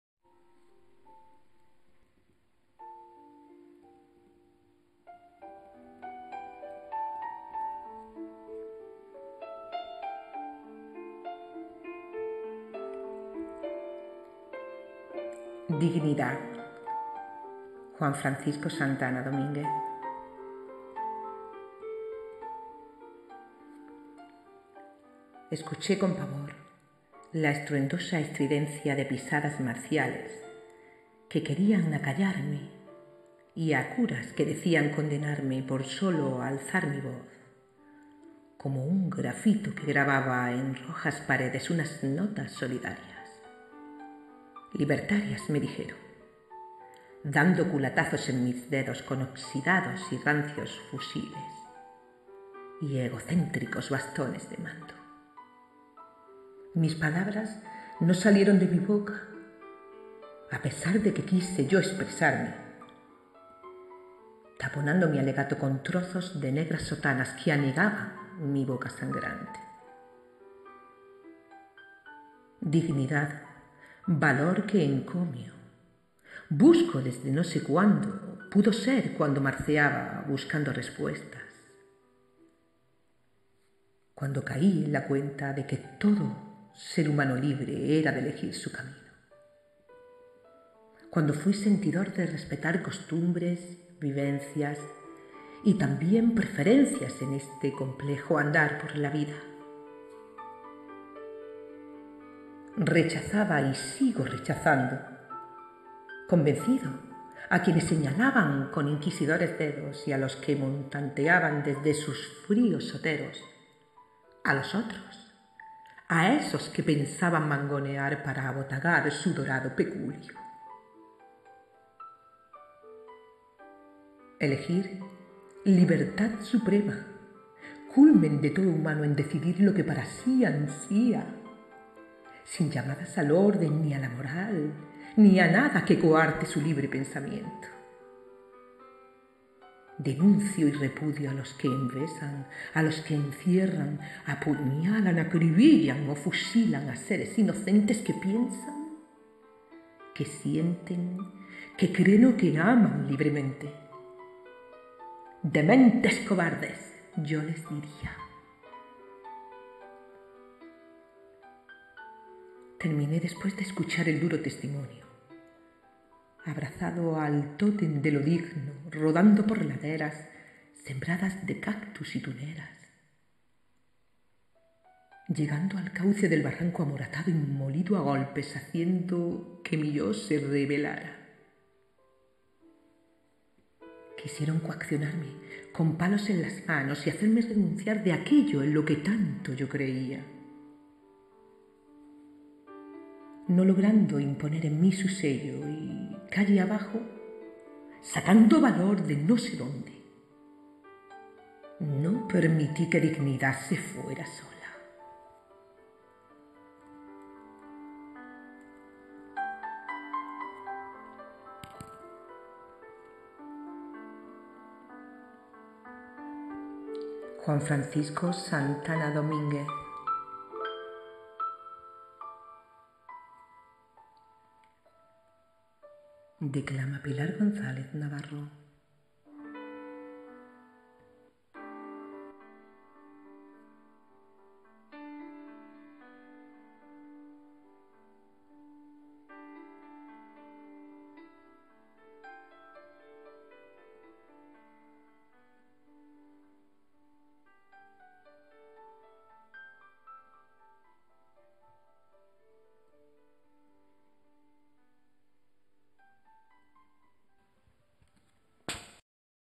"DIGNIDAD" de Juan Francisco Santana Dominguez.
Montaje y declamación: Pilar González Navarro.
DIGNIDAD

Escuché, con pavor, la estruendosa estridencia de pisadas marciales que querían acallarme y a curas que decían condenarme por sólo alzar mi voz como un grafito que grababa en rojas paredes unas notas solidarias, libertarias me dijeron, dando culatazos en mis dedos con oxidados y rancios fusiles y egocéntricos bastones de mando. Mis palabras no salieron de mi boca, a pesar de que quise yo expresarme, taponando mi alegato con trozos de negras sotanas que anegaban mi boca sangrante.
Dignidad, valor que encomio, busco desde no sé cuándo, pudo ser cuando marceaba buscando respuestas, cuando caí en la cuenta de que todo ser humano libre era de elegir su camino, cuando fui sentidor de respetar costumbres, vivencias y también preferencias en este complejo andar por la vida. Rechazaba y sigo rechazando, convencido, a quienes señalaban con inquisidores dedos y a los que montanteaban, desde sus fríos oteros, a los otros, a esos que pensaban mangonear para abotagar su dorado peculio.
Elegir, libertad suprema, culmen de todo humano en decidir lo que para sí ansía sin llamadas al orden, ni a la moral, ni a nada que coarte su libre pensamiento. Denuncio y repudio a los que envesan, a los que encierran, apuñalan, acribillan o fusilan a seres inocentes que piensan, que sienten, que creen o que aman libremente. ¡Dementes  cobardes! -yo les diría.

Terminé, después de escuchar el duro testimonio, abrazado al tótem de lo digno rodando por laderas, sembradas de cactus y tuneras, llegando al cauce del barranco amoratado y molido a golpes haciendo que mi yo se rebelara. Quisieron coaccionarme, con palos en las manos, y hacerme renunciar de aquello en lo que tanto yo creía, no logrando imponer en mí su sello y, calle abajo, sacando valor de no sé dónde, no permití que Dignidad se fuera sola.
Juan Francisco Santana Domînguez